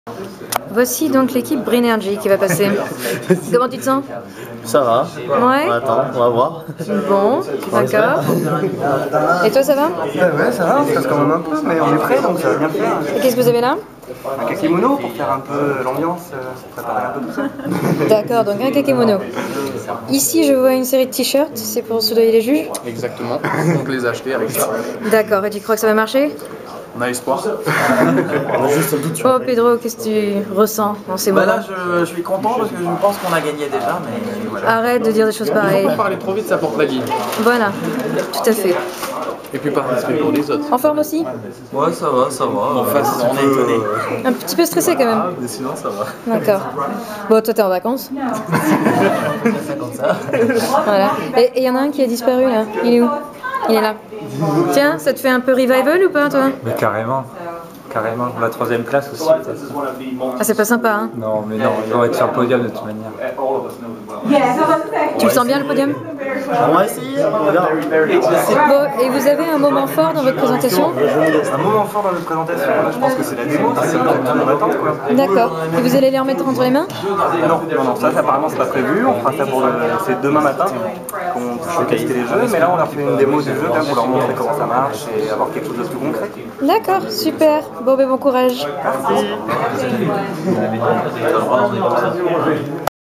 Imagine Cup 2011 - Brainergy -
Le stress monte juste avant le premier oral de l'équipe Geekologics (projet Brainergy) - Supinfogame & Ingesup - Catégorie Game Design Windows Phone 7